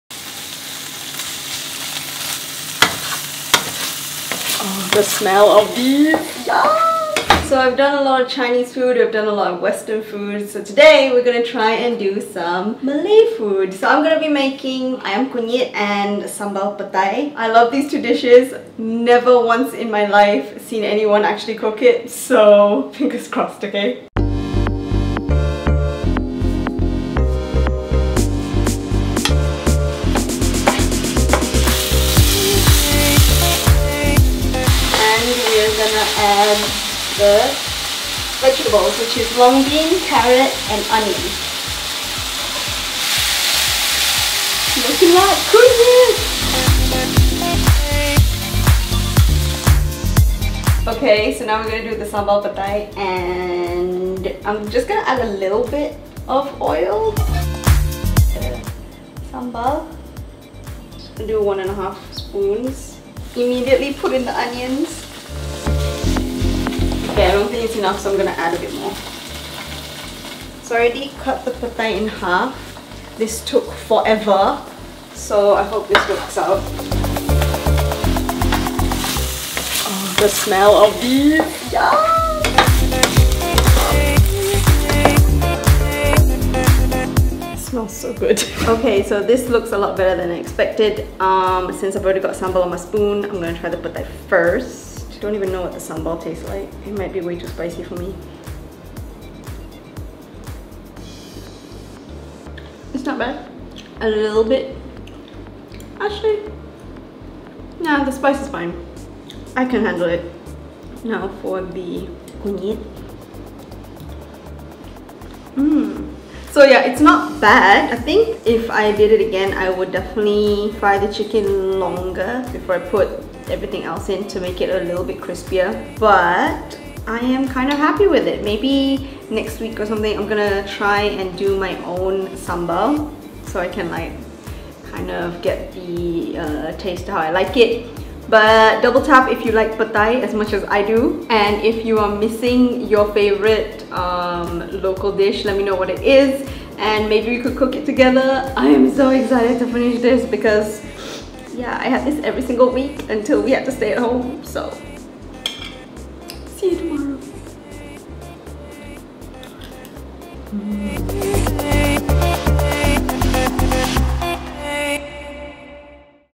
Ayam Kunyit & Sambal Petai [Calista Cooks #26] | HITZ CO-VIDS
It's time for some Malay food! Calista went out of her comfort zone and cooked one of her favourite dishes from before the MCO!